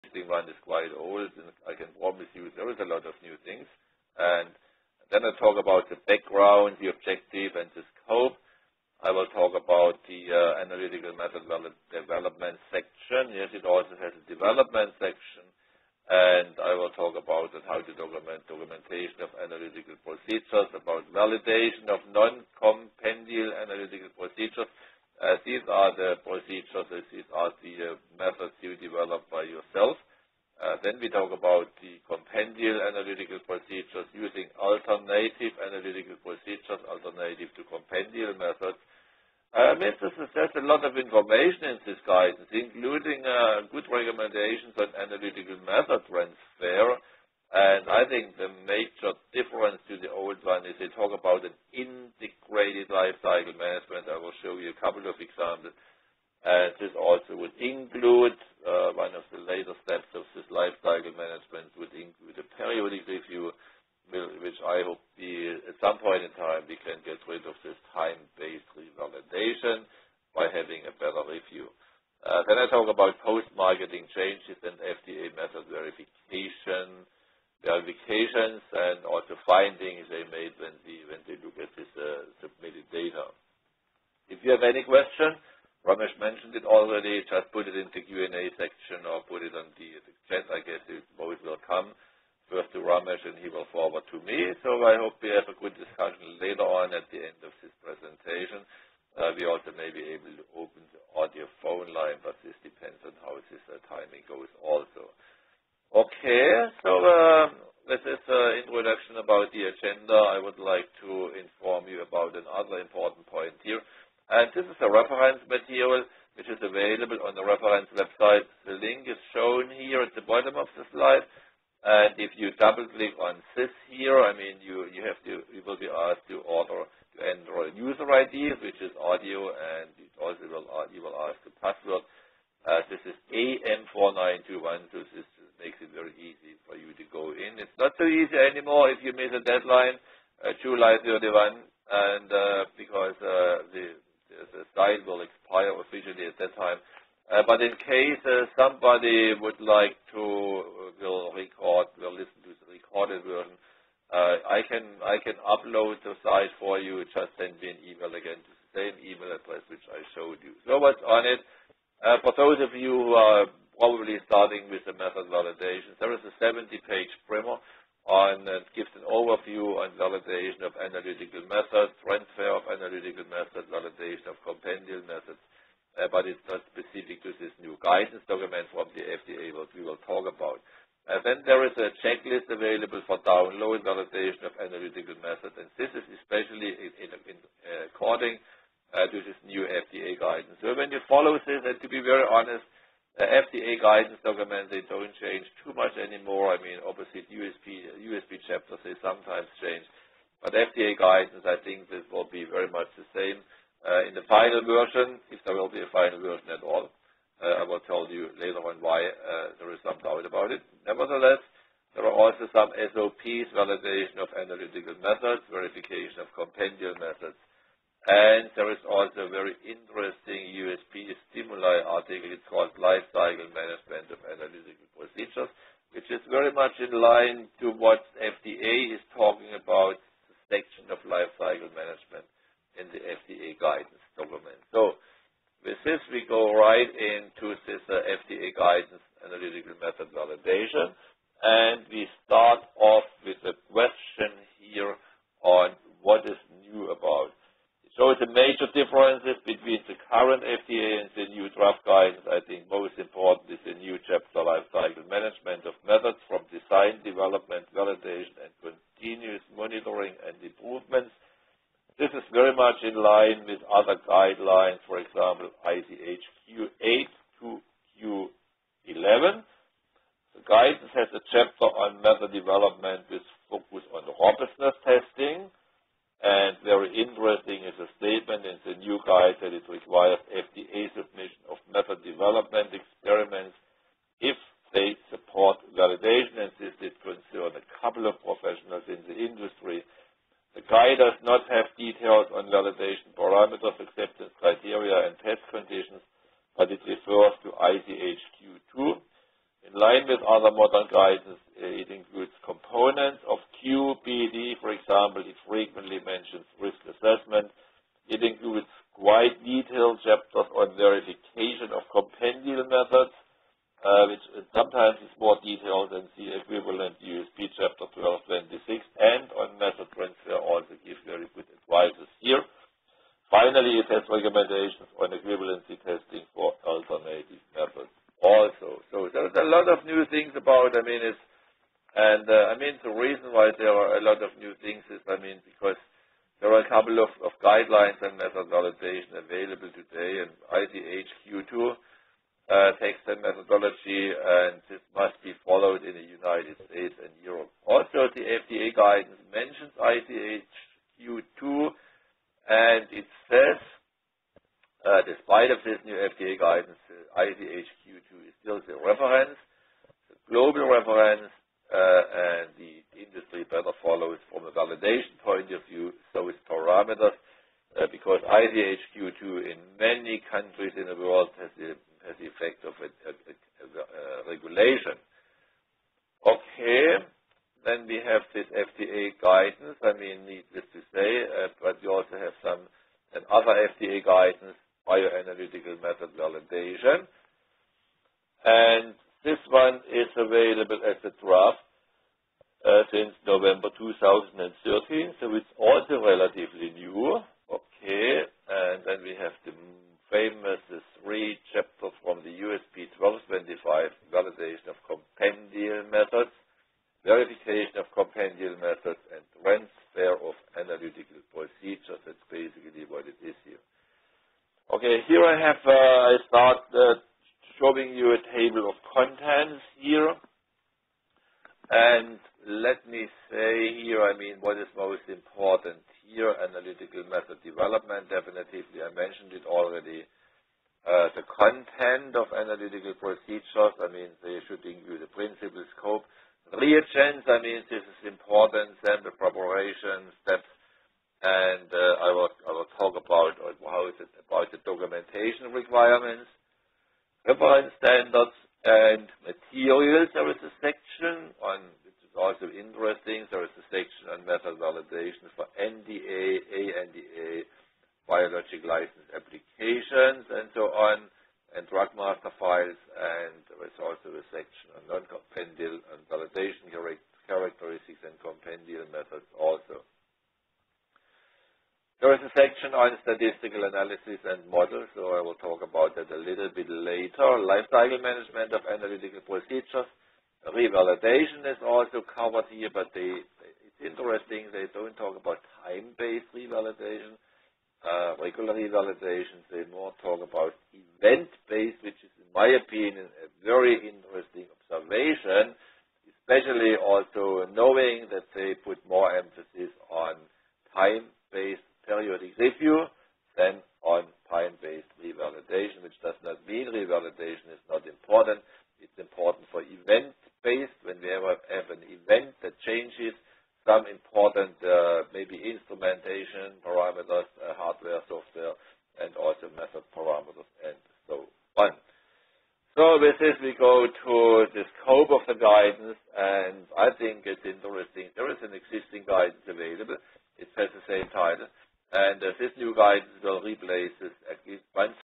Validation of Analytical Methods according to the New FDA Guidance
Recently the FDA has released a new comprehensive guidance for validation of analytical methods. The guidance applies the modern integrated lifecycle approach with related new requirements for using quality-by-design components, risk assessment, design space and continuous improvement. The guidance will significantly change the way in which FDA regulated methods will be developed, validated and used. Because of the novelty of some of the concepts, the industry has difficulties to implement the guidance. The seminar will make it easy to understand and implement the guidance.

For easy implementation, attendees will receive:

2 SOPs
Validation of Analytical Methods
Verification of Compendial Methods
Checklist